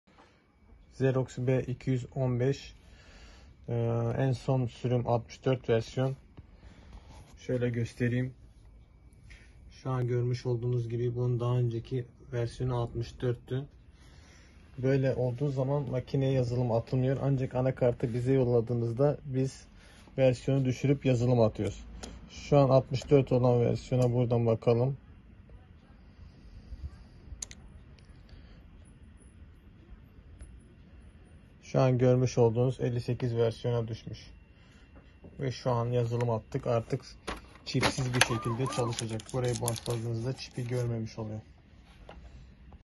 XEROX B215 YÜKSEK VERSİYON DÜŞÜRME
XEROX B215 YAZICININ ANAKARTINI BİZE YOLLARSANIZ SINIRSIZ  VE CHİPSİZ HALE  GETİRİYORUZ YUKSEK VERSİYON DÜŞÜRME
XEROX B215 VERSİYON DÜŞÜRME
XEROX B215 YAZILIM,
XEROX B215 ANAKART,
XEROX B215 RESET,
XEROX B215 TONER,